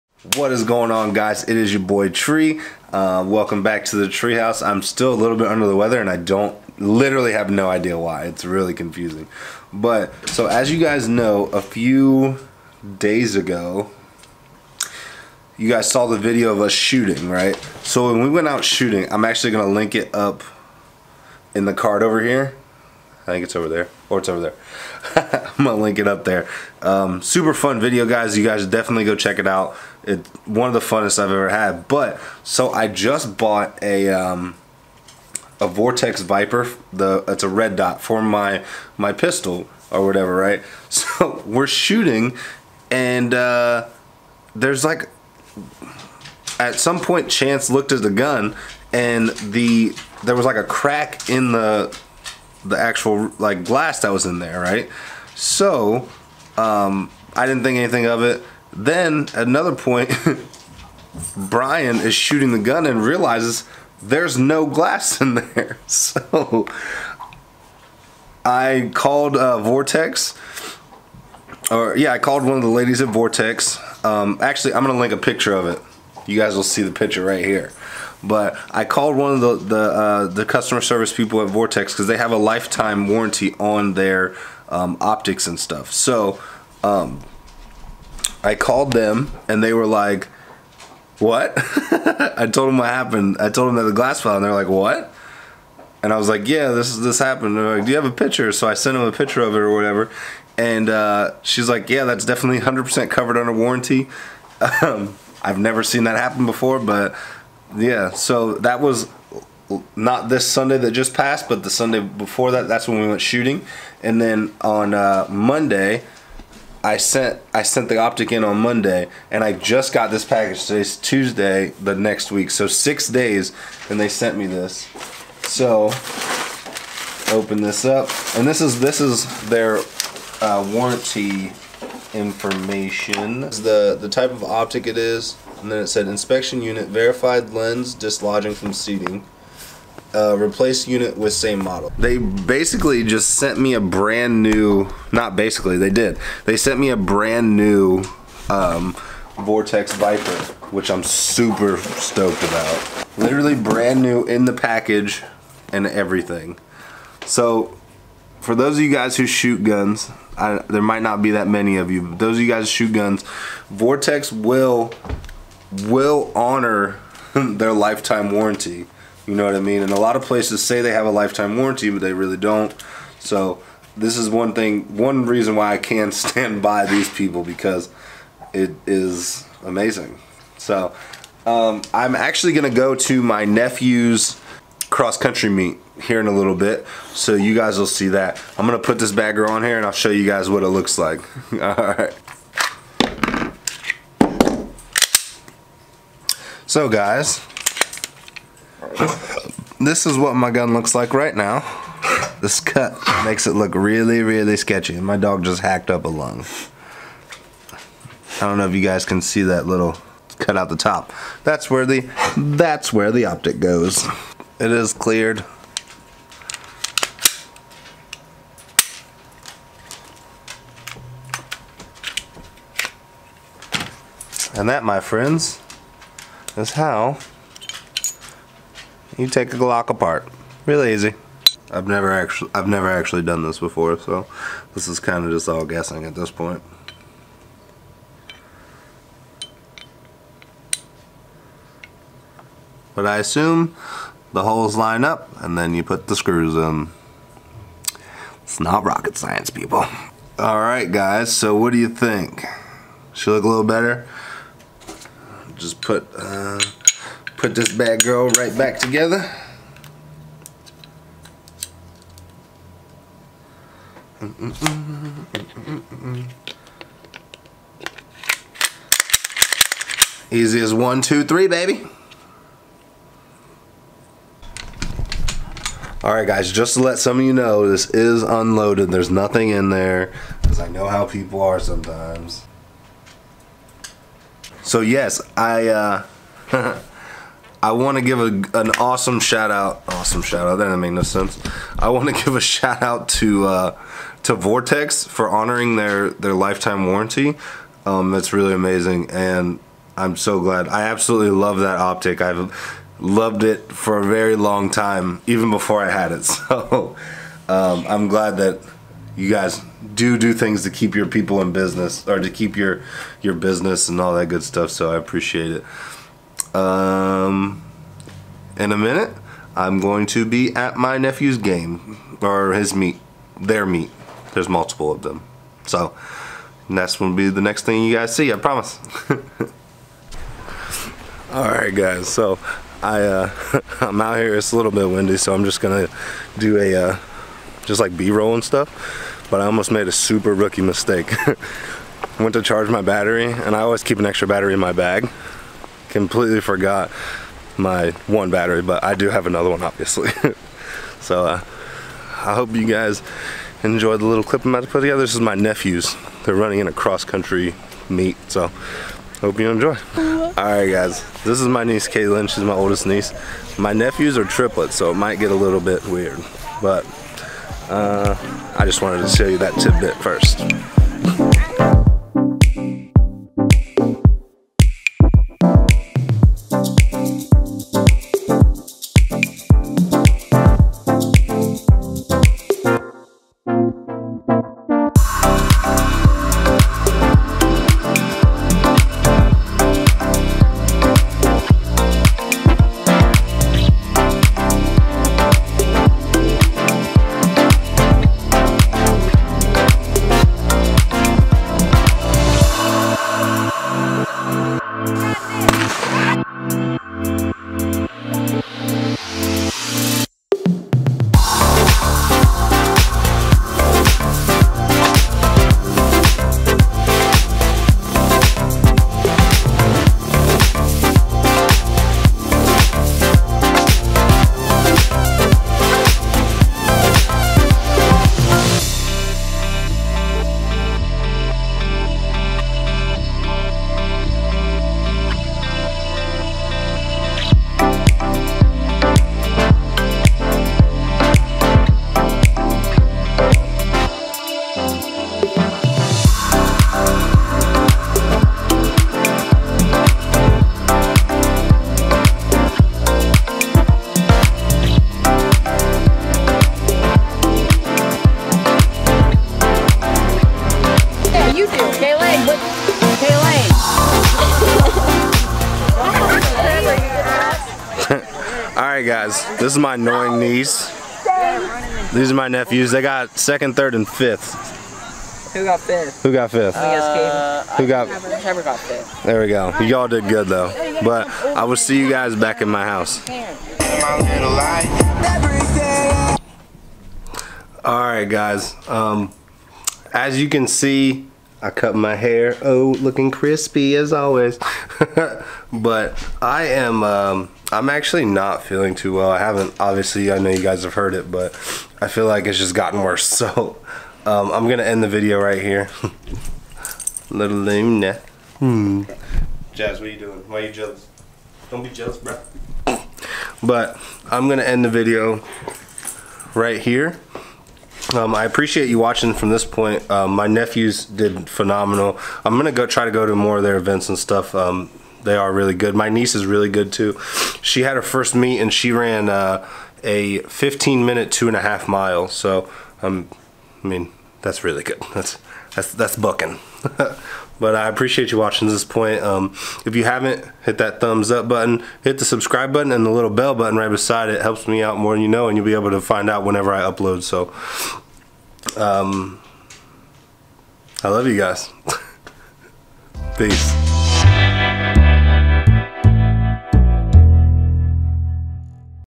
Does Vortex Optics Follow Through with Their Lifetime Warranty?
A little over a week ago I went shooting with my new Vortex Viper optic. If you have seen the video of that one then you know that the glass on my optic fell out. So I contacted vortex and asked if their lifetime warranty applied. They told me yes and had a shocking reaction to what had happened to the optic. After putting my optic back on I went to my nephews cross county meet and had a pretty cool edit that i put together for it. I hope that you guys enjoy this video! If you did you already know what to do.

Meal Prep Containers
Rice Cooker
Food Scale
DJi Spark
Canon SL2
Rode VideoMicro